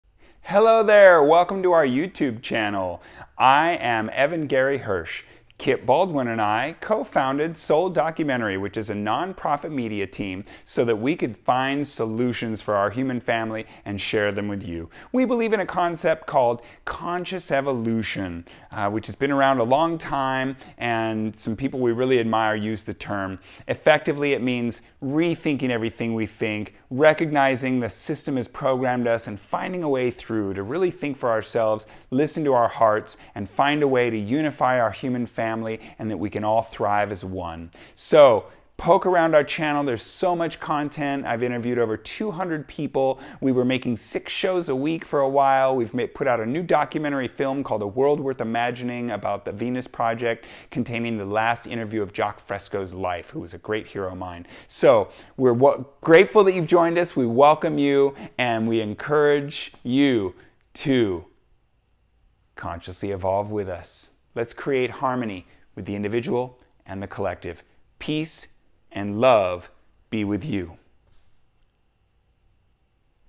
Welcome to the LOVE Paradigm - S.O.U.L. Documentary YouTube welcome video
S.O.U.L. founder Evan Gary Hirsch offers a greeting and introduction to our non-profit media team, who are here to promote conscious evolution and harmony for the individual and the collective human family. Time to thrive!